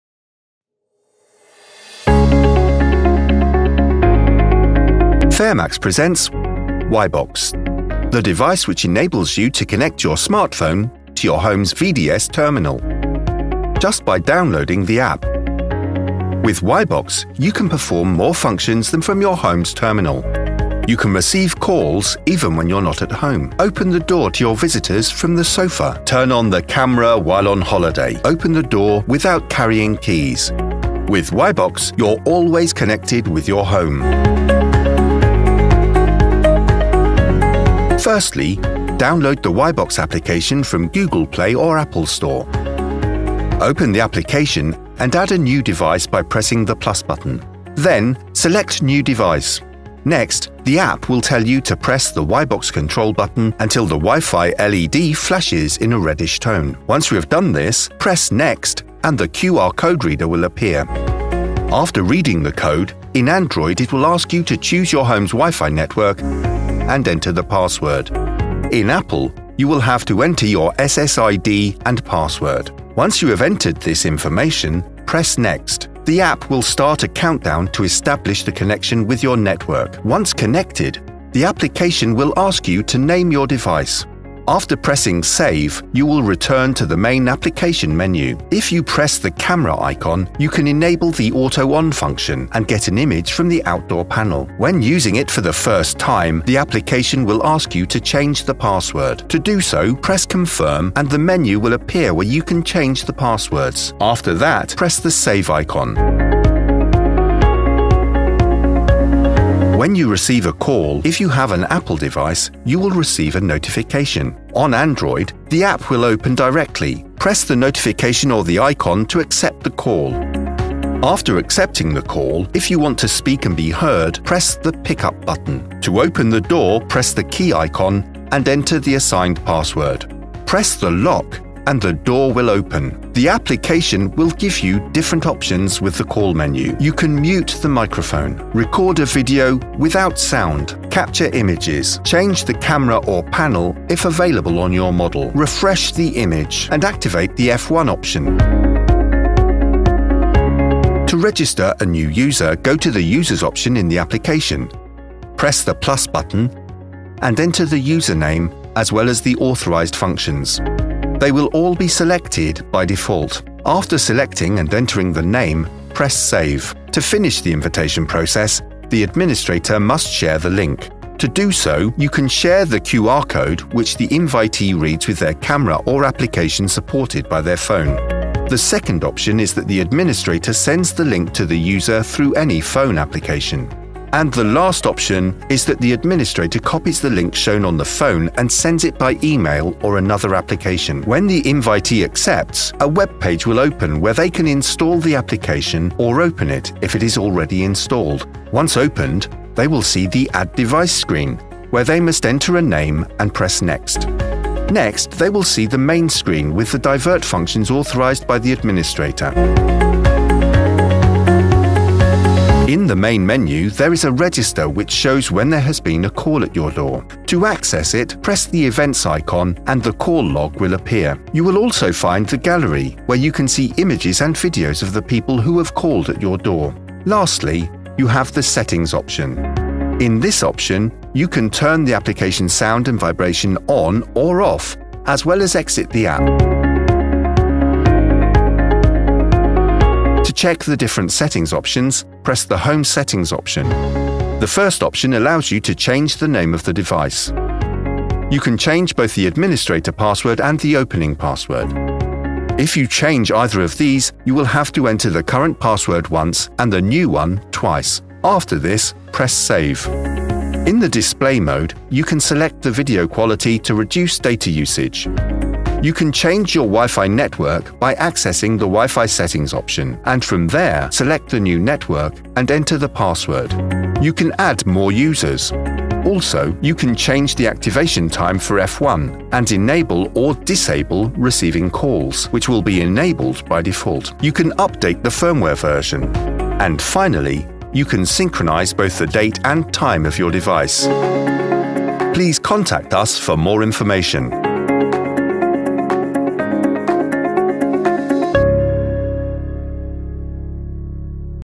[TUTO] WI-BOX | How to pair your smartphone to a VDS monitor or telephone | FERMAX
Fermax presents WI-BOX (Ref. 3266), the module that allows you to  connect your mobile phone to the VDS terminal in your home, just by downloading the APP.

With WI-BOX you will can perform the functions that you normally fo from your home's terminal and much more!

WI-BOX is compatible with FERMAX VDS terminals, both telephones and monitors, no matter how old your equipment is.

Follow this video-tutorial to pair your FERMAX VDS door phone or door entry monitor to your mobile through your home's WiFi Internet connection. You will also find information about the use of other functions:
- Receive calls.
- Open the door from your mobile.
- Auto-on the outdoor panel camera.
- Change the opening and administrator passwords.
- Mute the call, change panels, adjust image refreshment, etc.
- Capture images or videos.
- Add new users.